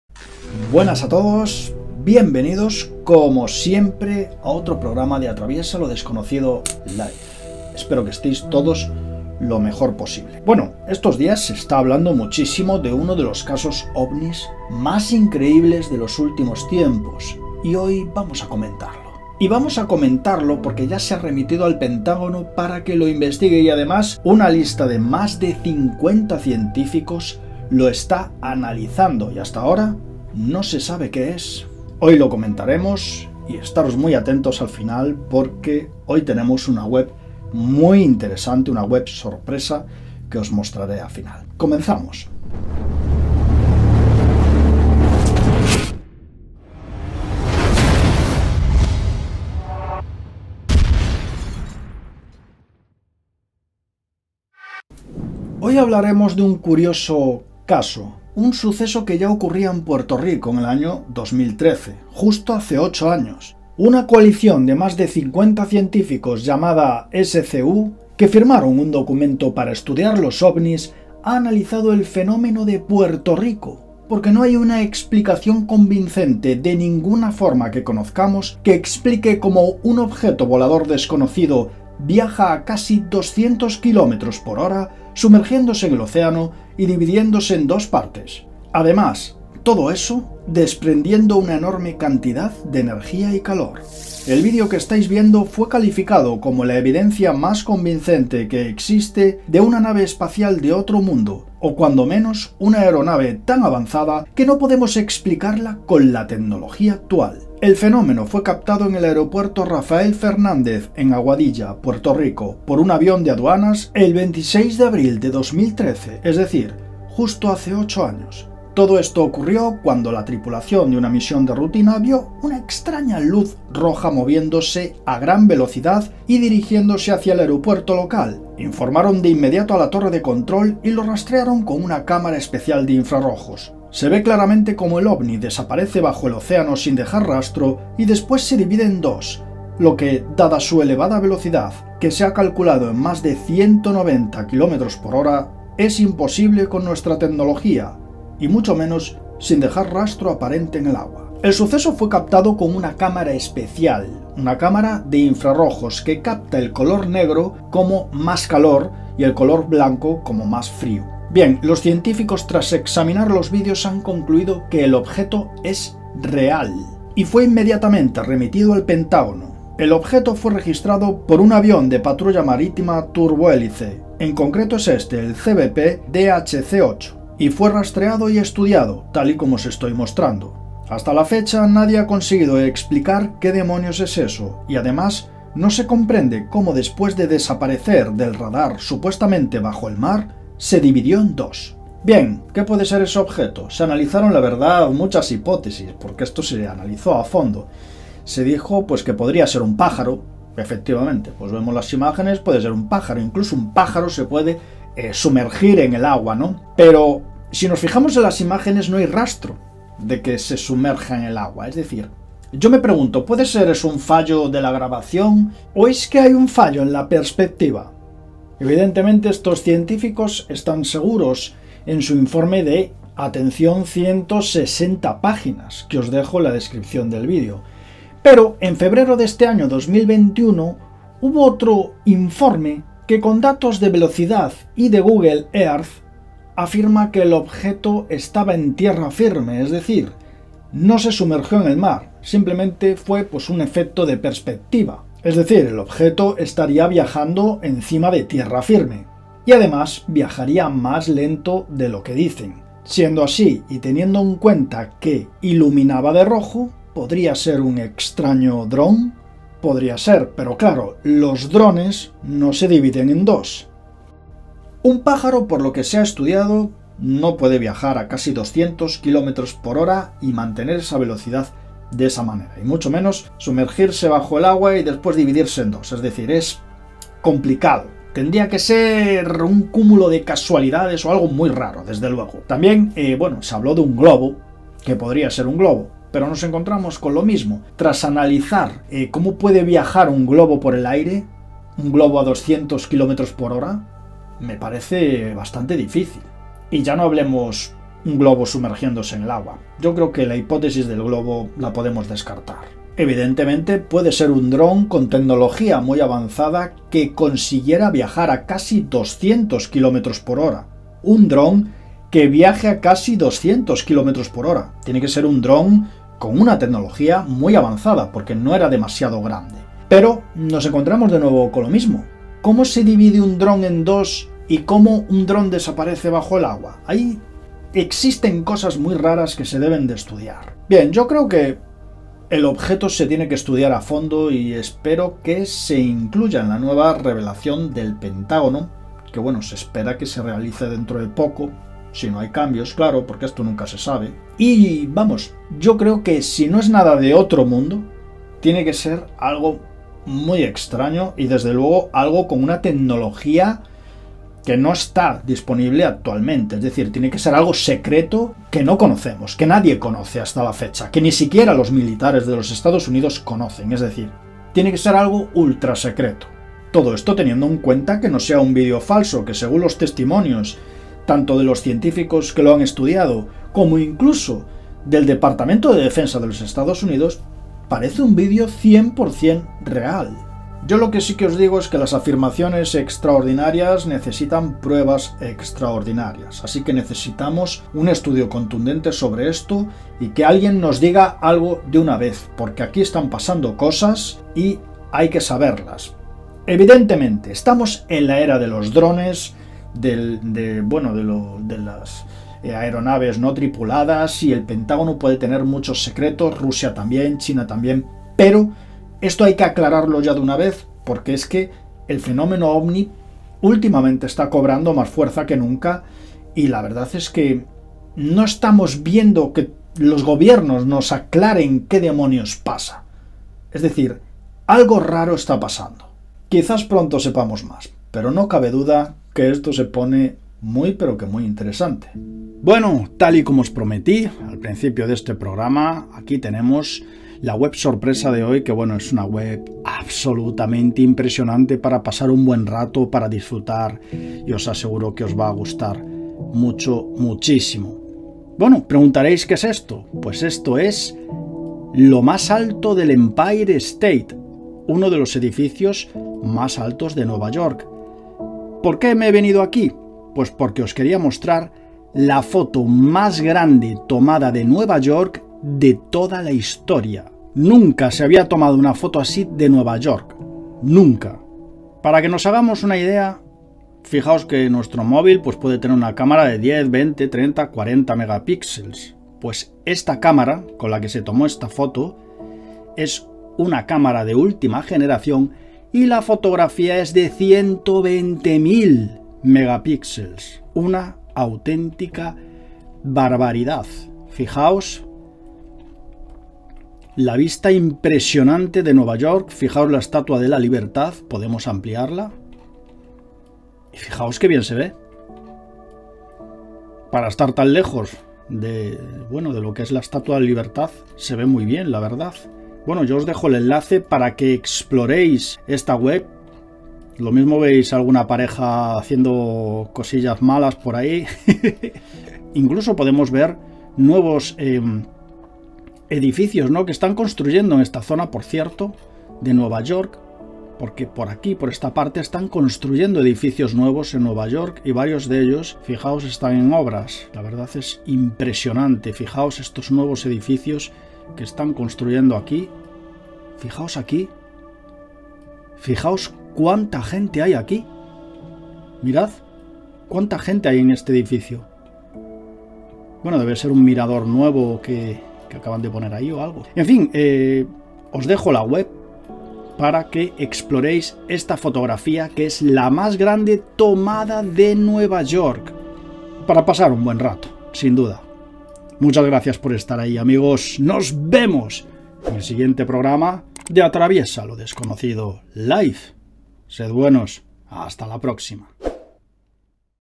INEXPLICABLE! Objeto que se Dividió en DOS por Encima de Puerto Rico es Investigado por Científicos
Numerosos científicos no entienden que ha pasado con un Objeto Volador No Identificado capturado volando sobre Puerto Rico, ya que el objeto en cuestión se divide en dos partes y parece sumergirse en el océano, todo a una velocidad asombrosa… ¿Qué es ese objeto? ¿Qué afirman los científicos que lo estudian?